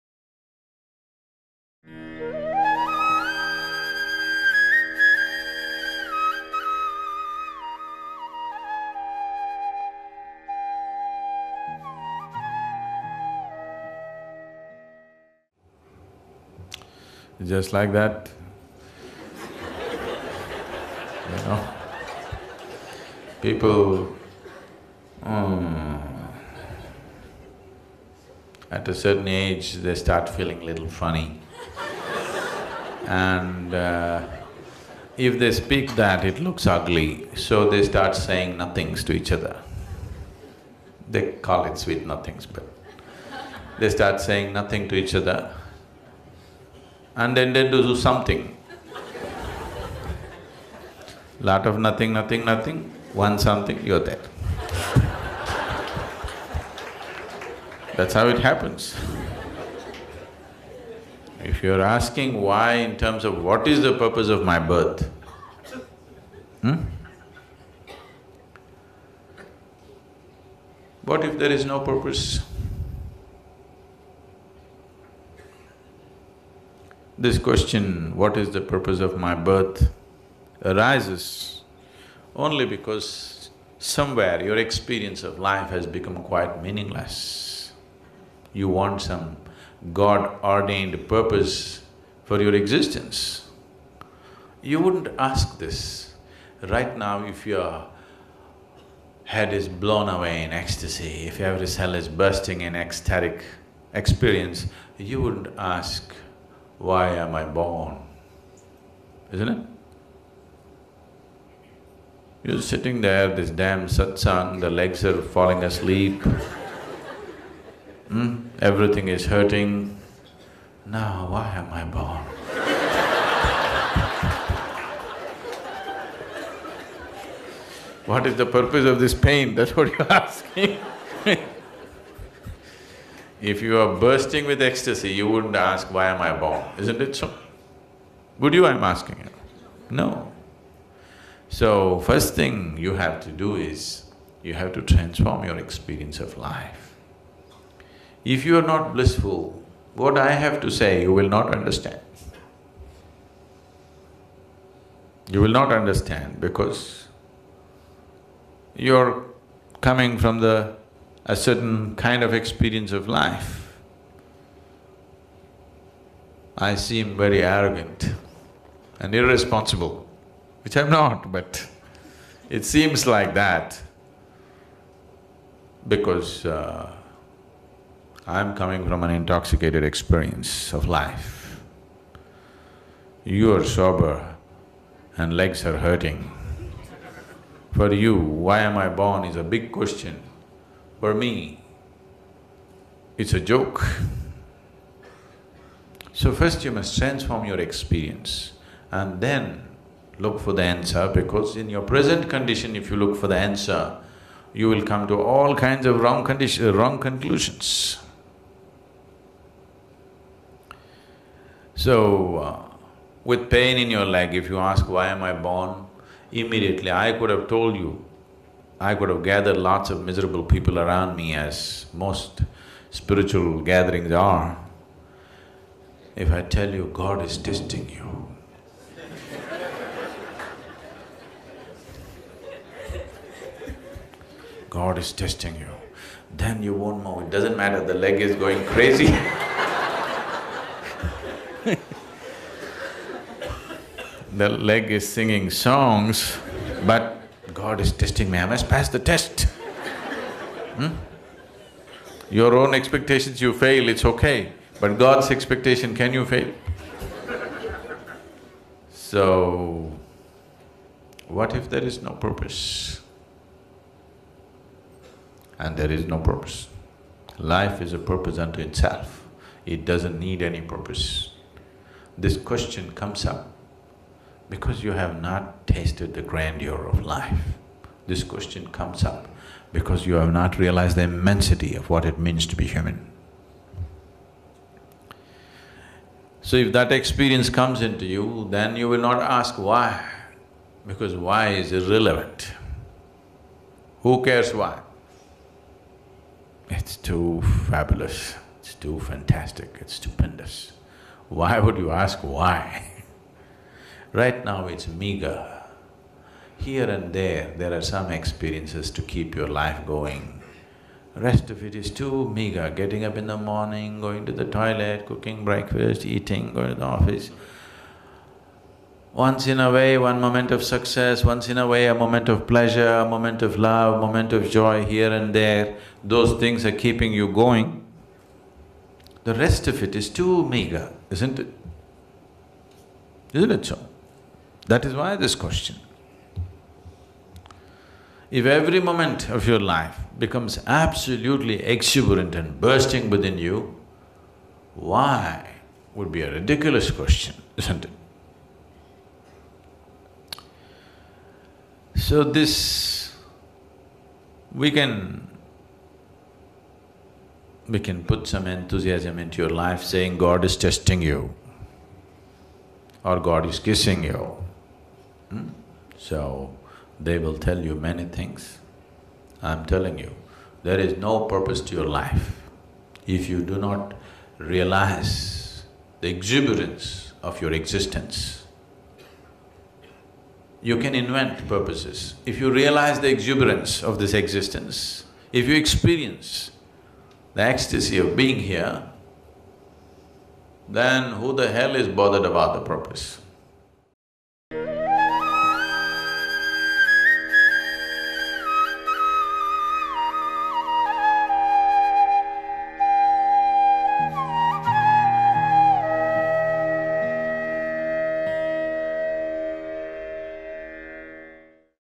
我来此生的目的何在？| Sadhguru 萨古鲁
萨古鲁回答了一个问题，并剖析了只是因为对生命的体验不够丰盈，有关生命的目的的问题才会产生。萨古鲁说：“如果你没有意识到你的存在的生机勃勃，你会编造目的。如果你体验到在这里存在的狂喜，有谁会在意目的呢？”